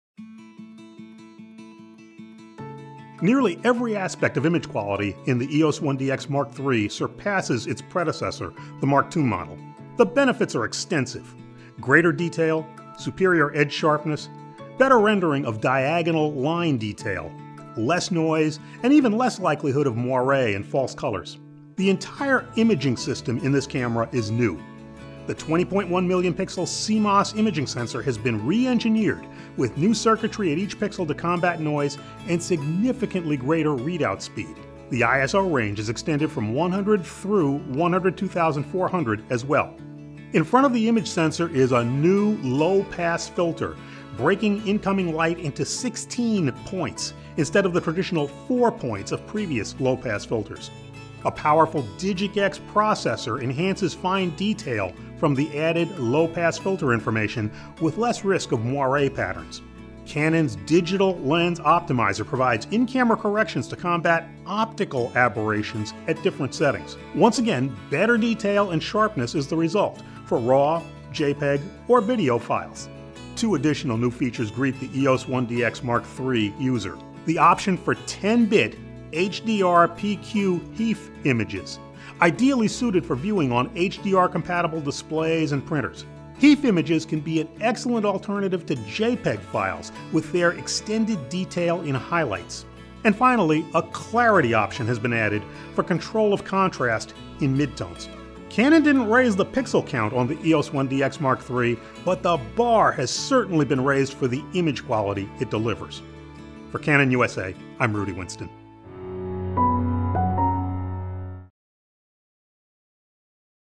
Canon EOS-1D X Mark III Image Quality
Learn about Canon’s EOS-1D X Mark III’s powerful new DIGIC X image processor and re-engineered 20.1 million pixel CMOS imaging sensor to provide incredible detail and image quality